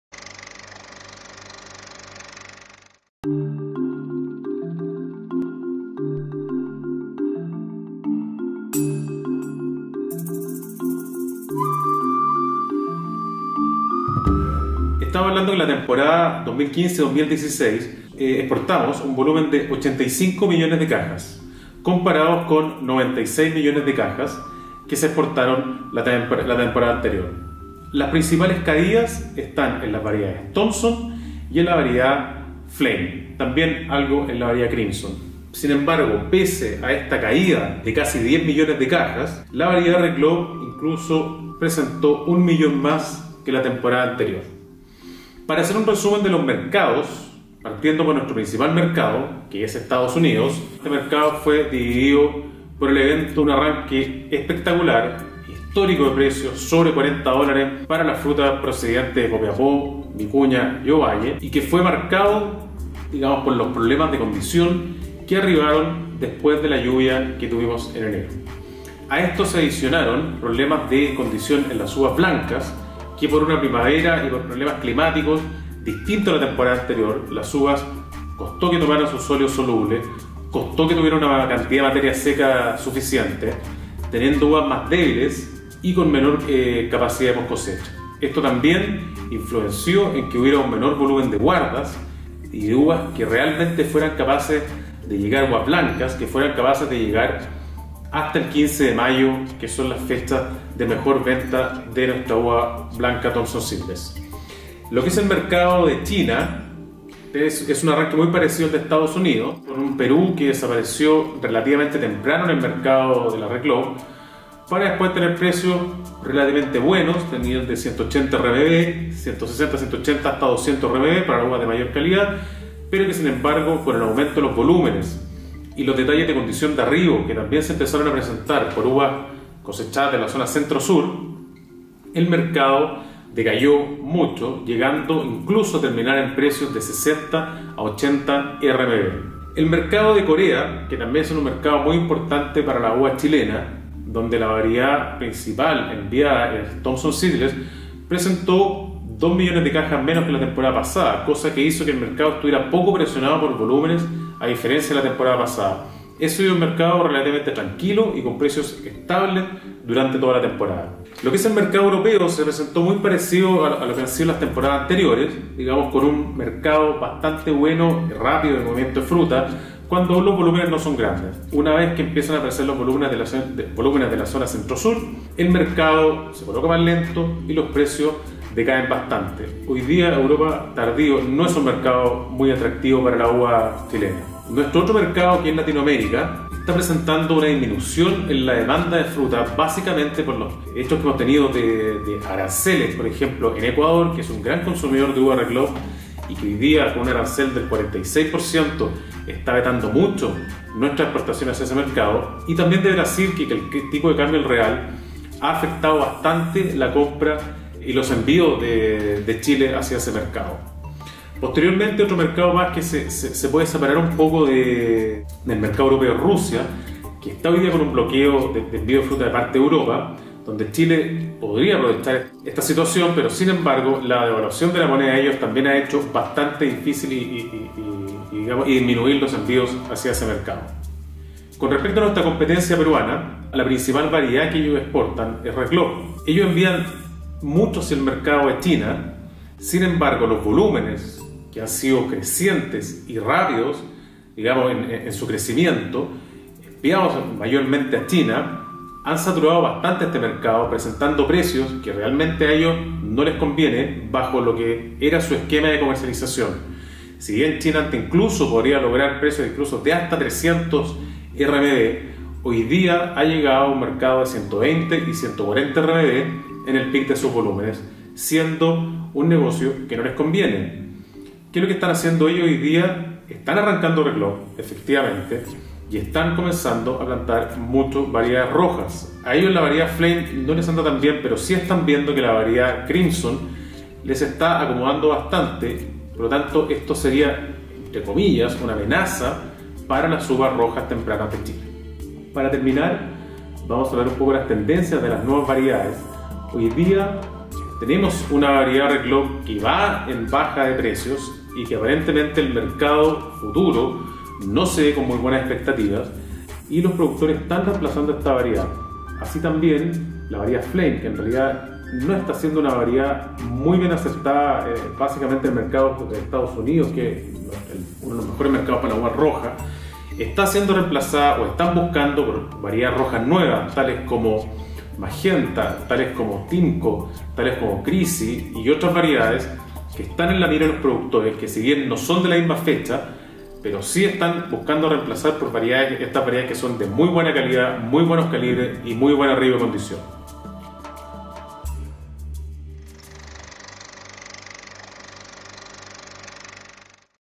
RESUMEN COMERCIAL TEMPORADA 2015/ 16 UVA DE MESA
El Ing. Agr. René Flores (UDECHILE), nos entrega un resumen comercial de los aspectos más importantes que afectaron la temporada 2015/ 16 de uva de mesa.